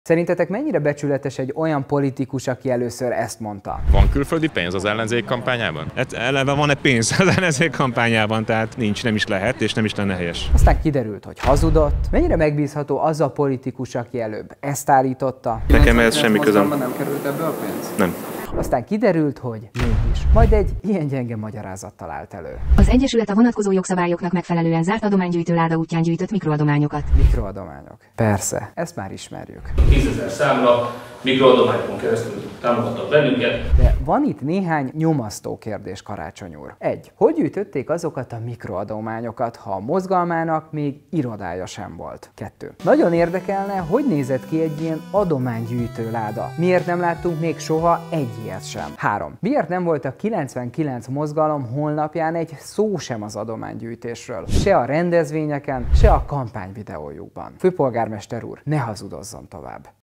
SZERINTETEK Ő becsületes politikus?
Karácsony Gergely azt állítja, hogy az 500 millió forint, amelyet gyanús körülmények között kapott mikróadományok voltak. Lebukott!